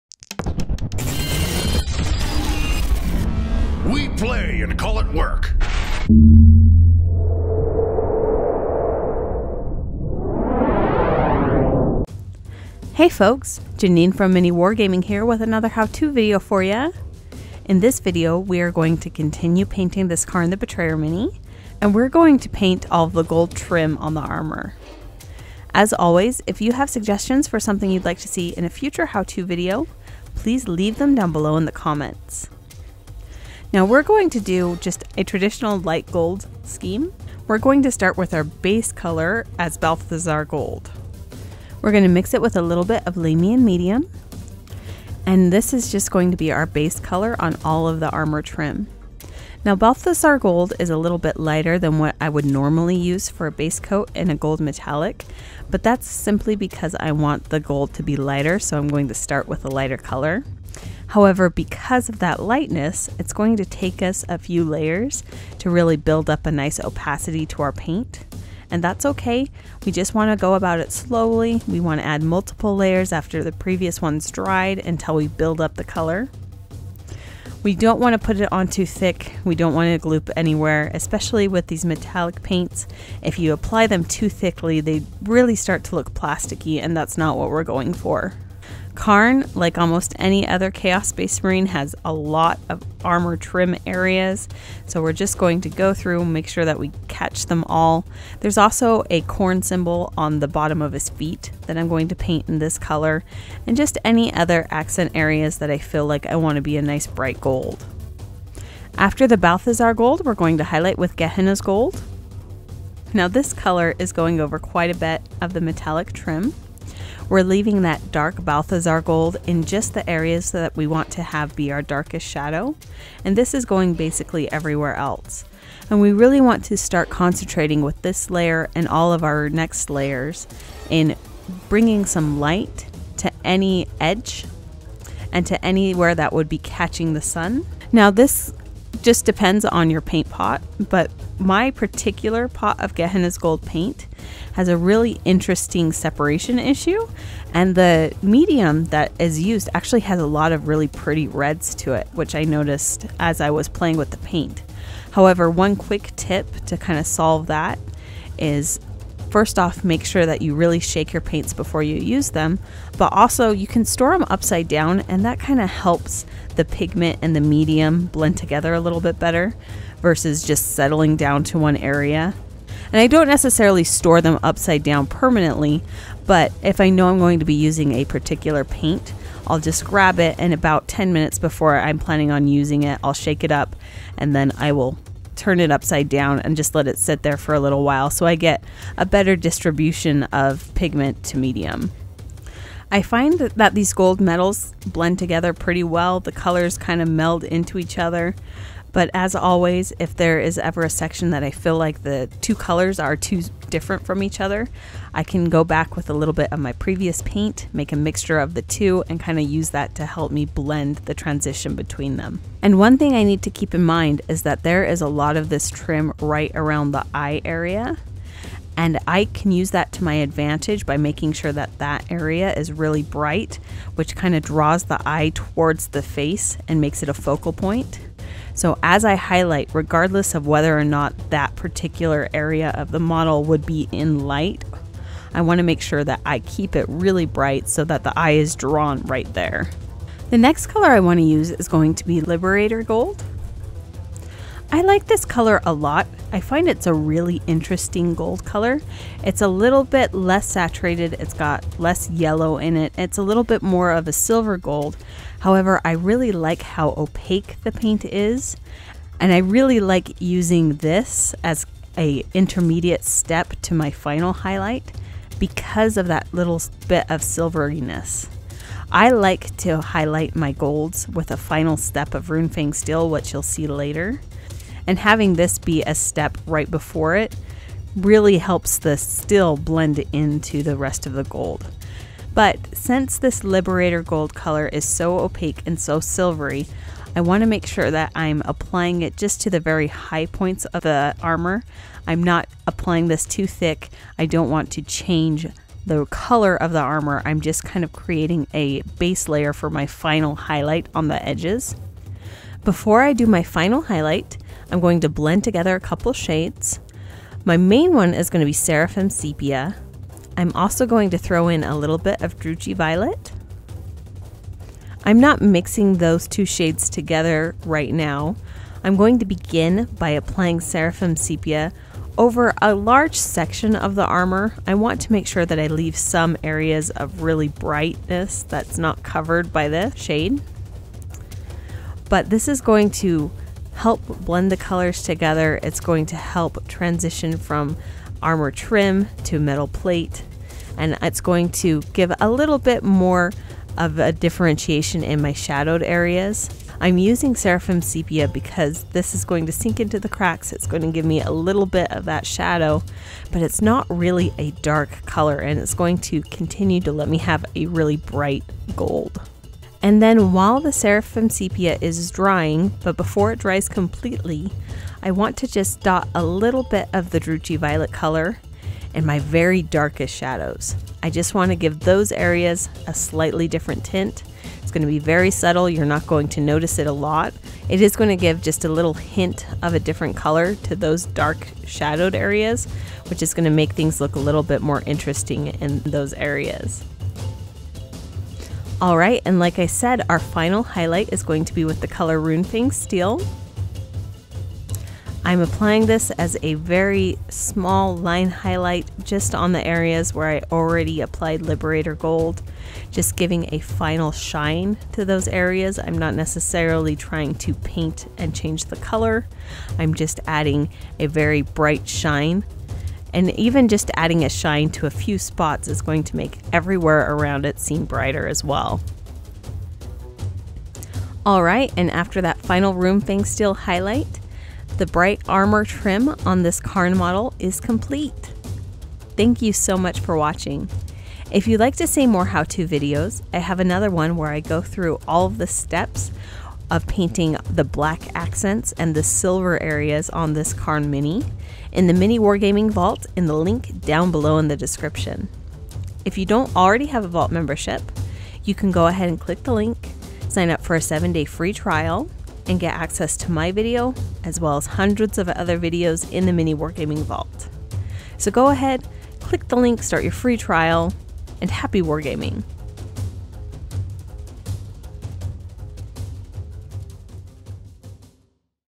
How To: Paint Basic Gold Armor Trim
In this video Janene shows you how to paint a basic bright gold armor trim on Kharn the Betrayer.